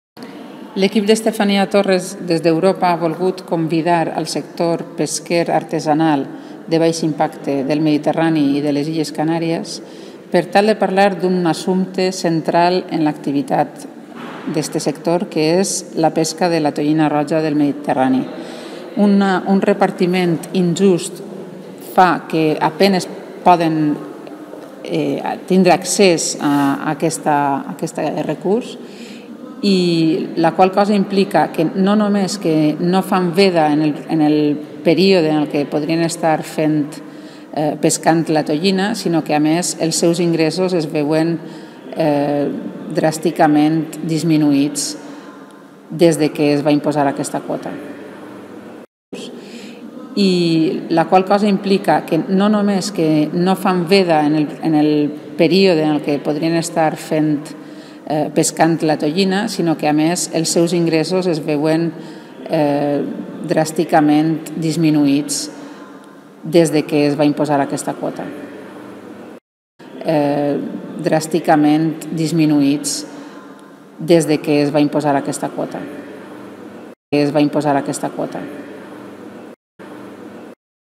Podemos recoge en el Grao las peticiones de los pescadores artesanales
Podemos organizó este fin de semana una Asamblea Abierta de la Pesca Artesanal en el Grao de Castelló con el fin de escuchar las peticiones del sector y así trabajar en la mejora de sus condiciones de vida. La Asamblea reunió a más de 150 pescadores y pescadoras venidos de Andalucía, Canarias, Murcia, Cataluña y la Comunidad Valenciana así como a representantes de Greenpeace y de la asociación LIFE (Low Impact Fishers of Europe).